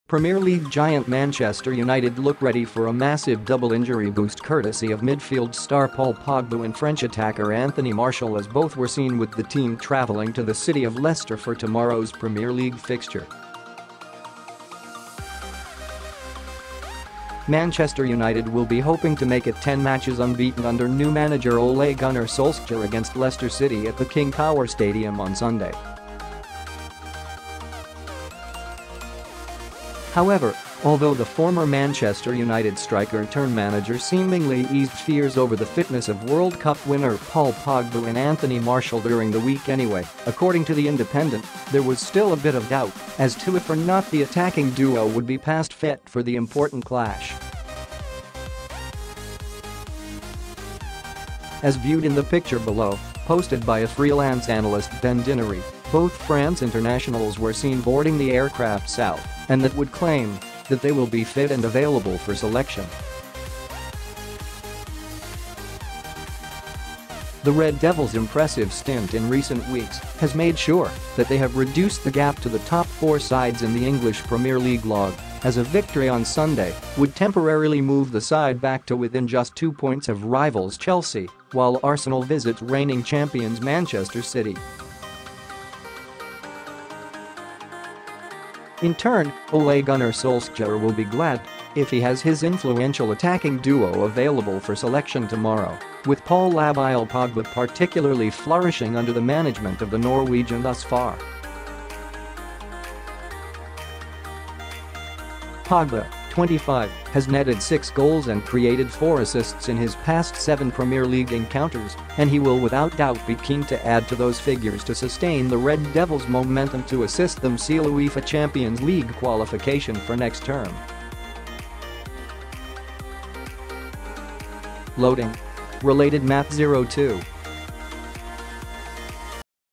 {PHOTO} GOOD NEWS!!! Man United receive DOUBLE injury boost as Superstar pair travel with squad for
Premier League giant Manchester United look ready for a massive double injury boost courtesy of midfield star Paul Pogba and French attacker Anthony Martial as both were seen with the team travelling to the City of Leicester for tomorrow’s Premier League fixture.  Manchester United will be hoping to make it 10 matches unbeaten under new manager Ole Gunnar Solskjaer against Leicester City at the King Power Stadium on Sunday.  However, although the former Manchester United striker turn manager seemingly eased fears over the fitness of World cup winner Paul Pogba and Anthony Martial during the week anyway, according to The Independent, there was still a bit of doubt as to if or not the attacking duo would be passed fit for the important clash.  As viewed in the picture below, posted by a freelance analyst Ben Dinnery, both France internationals were seen boarding the air craft south, and that would claim that they will be fit and available for selection.   The Red Devil’s impressive stint in recent weeks has made sure that they have reduced the gap to the top four sides in the English Premier League log, as a victory on Sunday would temporarily move the side back to within just two points of rivals Chelsea while Arsenal visit reigning champions Manchester City.  In turn, Ole Gunnar Solskjaer will be glad if he has his influential attacking duo available for selection tomorrow, with Paul Labile Pogba particularly flourishing under the management of the Norwegian thus far.  Pogba, 25, has netted six goals and created four assists in his past seven Premier League encounters, and he will without doubt be keen to add to those figures to sustain the Red Devil’s momentum to assist them seal UEFA Champions League qualification for next term.     Loading...  Related Matt 02